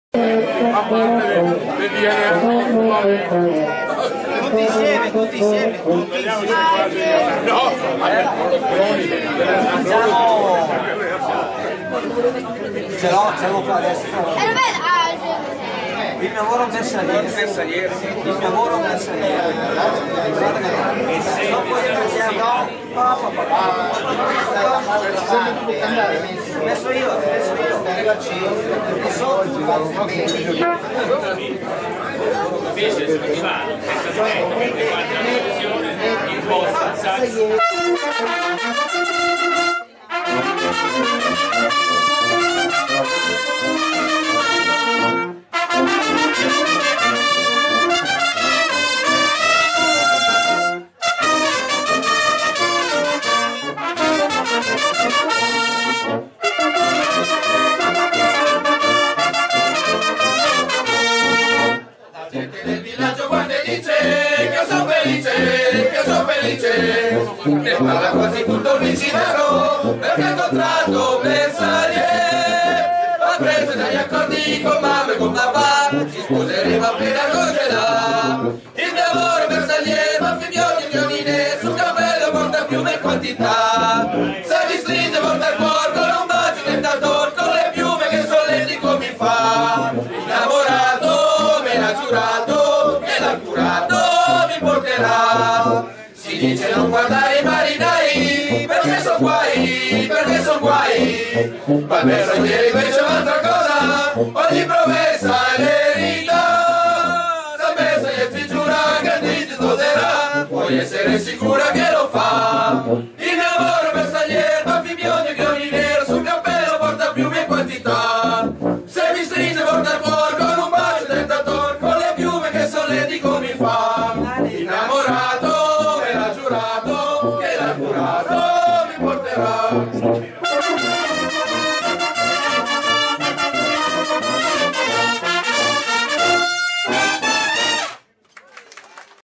dopo l'ALZABANDIERA per l'11° Reggimento Bersaglieri - by Giovanni Rosin - John
... video dedicato al mio Amico Franco Vidotto,
La manifestazione "ALZABANDIERA per l'11° Reggimento Bersaglieri" si è svolta il 17 marzo 2013 presso il Museo Storico Militare - Culturale "Casa del Bersagliere" di Jesolo.